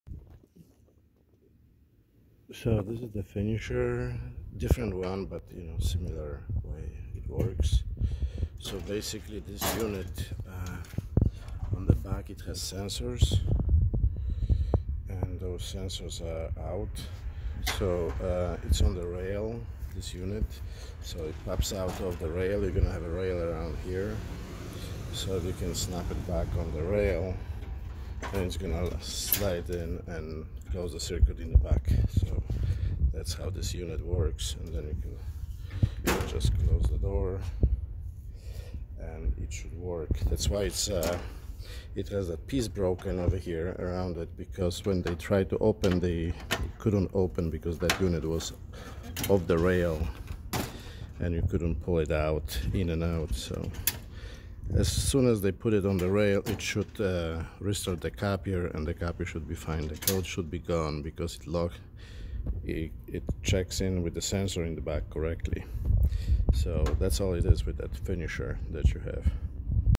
Finisher problem
You can pull it out and lift it up so it will snap back on the rails.
Make sure it goes in and out smoothly. Restart the copiers and the code should be gone.